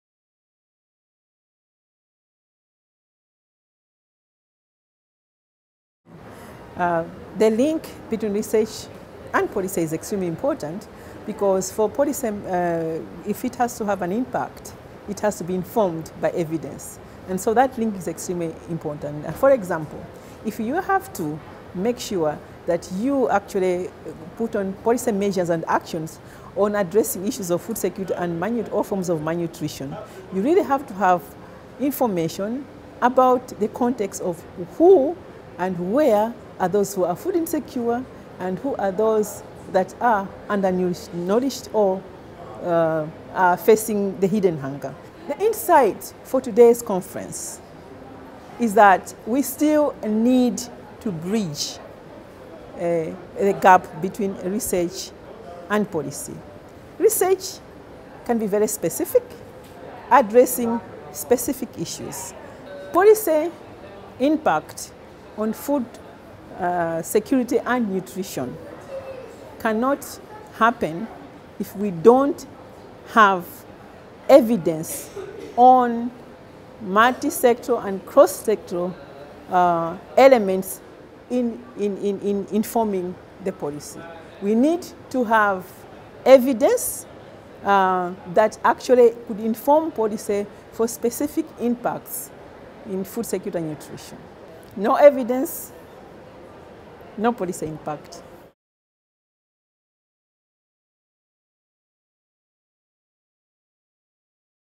Research & policy: two peas in a pod? Interview with Brave Ndisale
Conference 1 December 2017 "Research & policy: two peas in a pod? A dialogue for food security impact"
Interview with Brave Ndisale
(Video by The Broker)